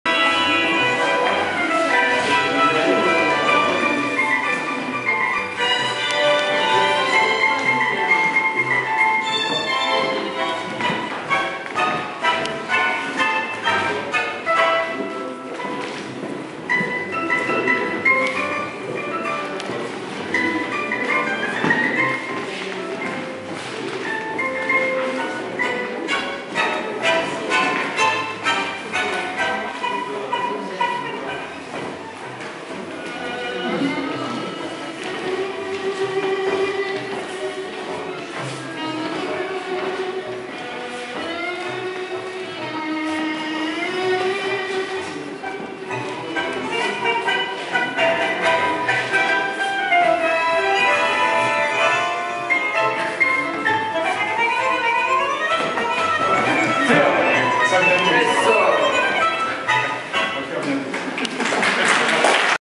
resume cours tangueando+06 dec 2012+-cesar et virginia.MP4
Resumé du cours avancé jeudi soir à Tangueando Toulouse
maestros Virginia Uva y Cesar Agazzi